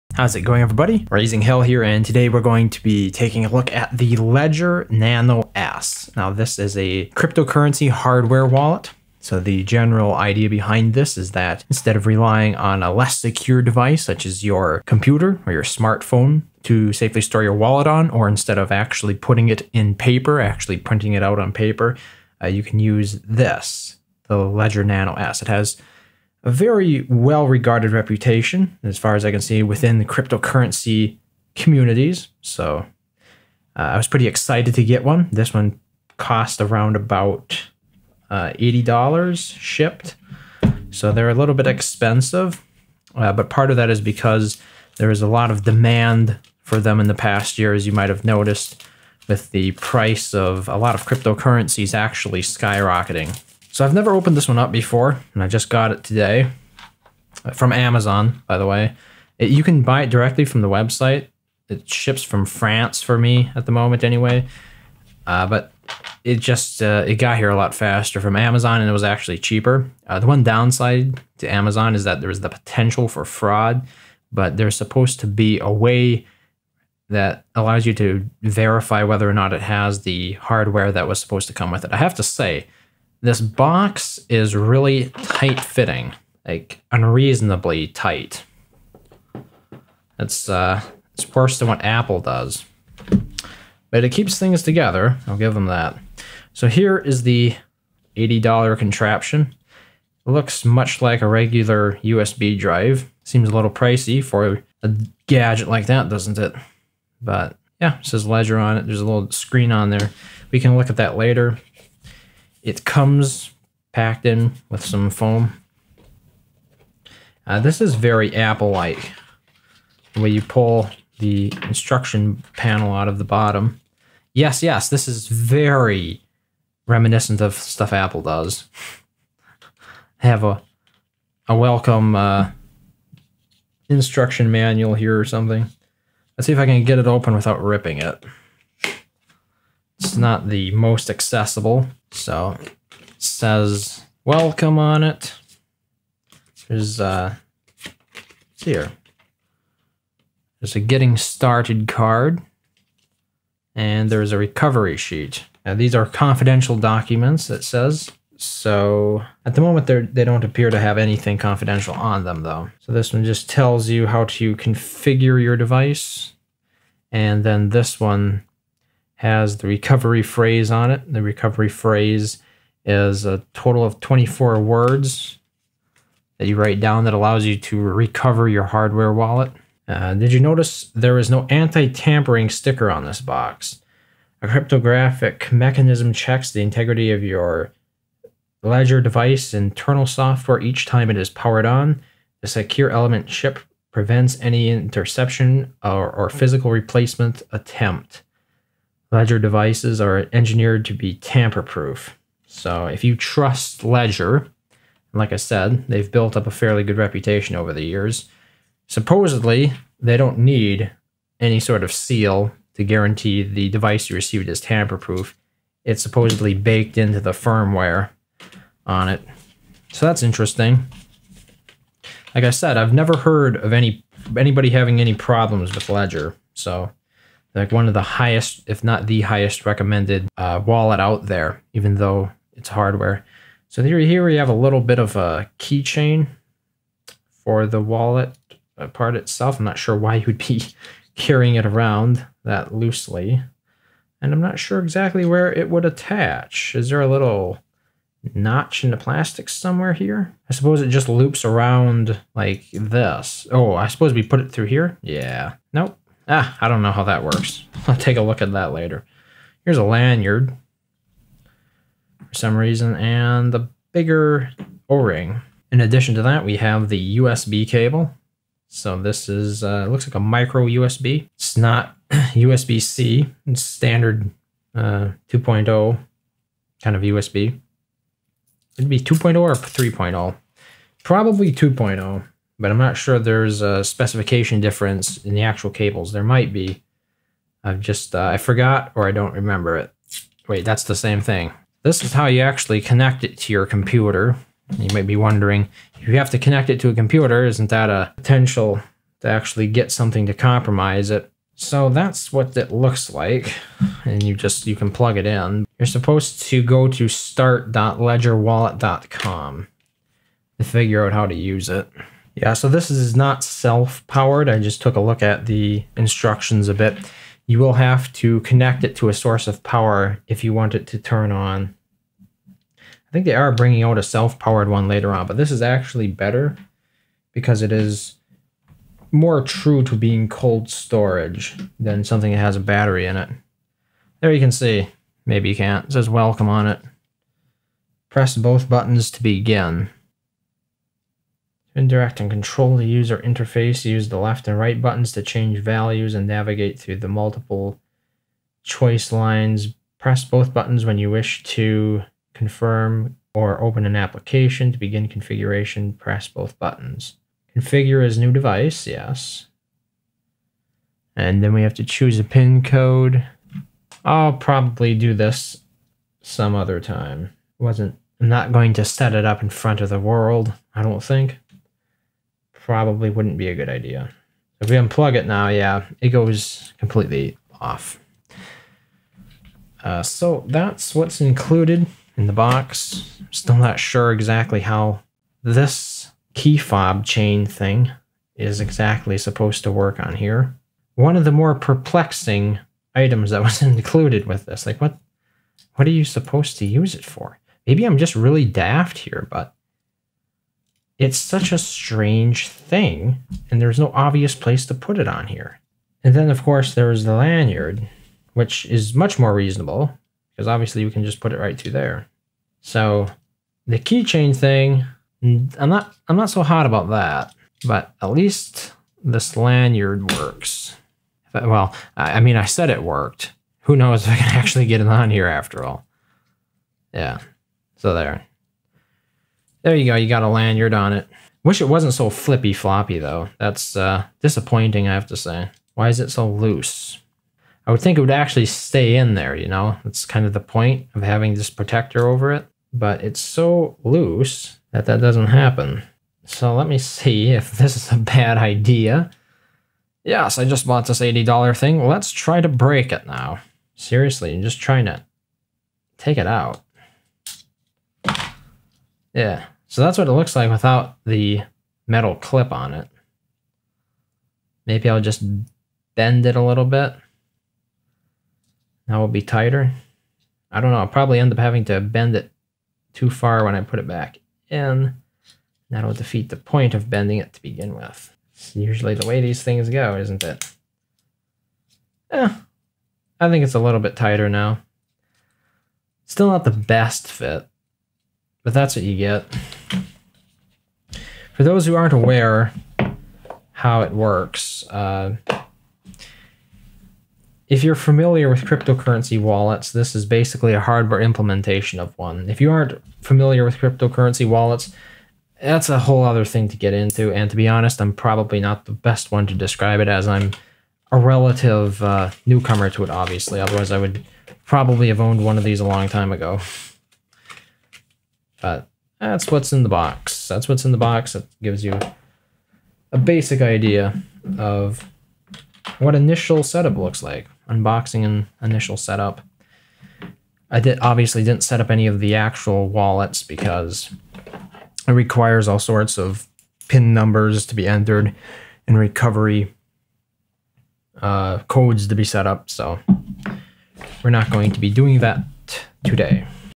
Unboxing the LEDGER NANO S Bitcoin/Cryptocurrency Hardware Wallet
Today I felt like unboxing and talking about the cryptocurrency hardware wallet from Ledger called the Ledger Nano S.

You can get a Ledger Nano S from Amazon